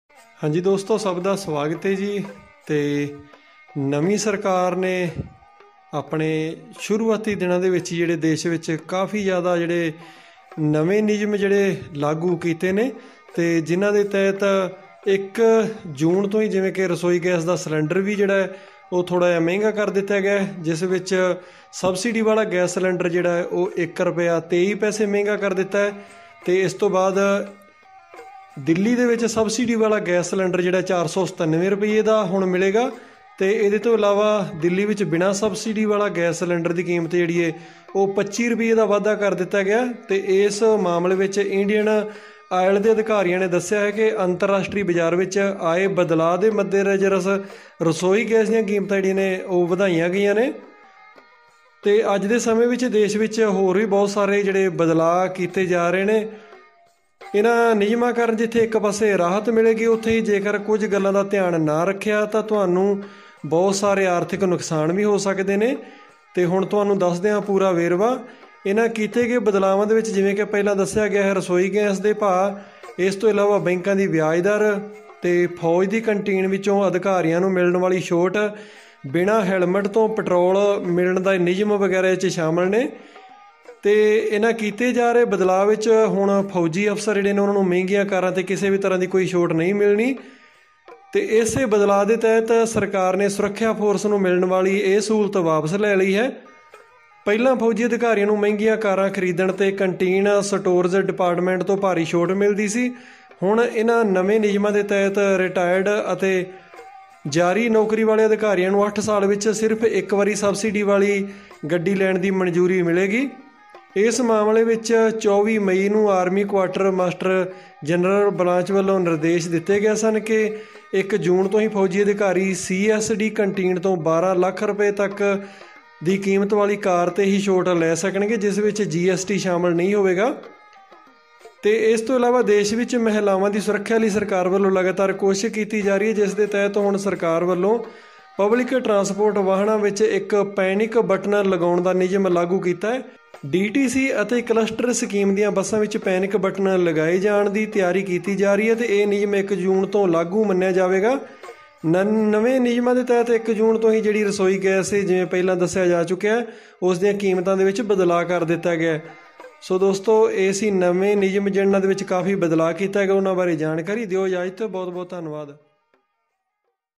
ਮੌਦੀ ਸਰਕਾਰ ਨੇ ਅੱਜ ਤੋ ਬਦਲੇ ਨਿਯਮ ਆਰਥਿਕ ਨੁਕਸਾਨ ਤੋਂ ਬਚੋ | PiTiC Live
Center government change some policies
Pls aware about new changes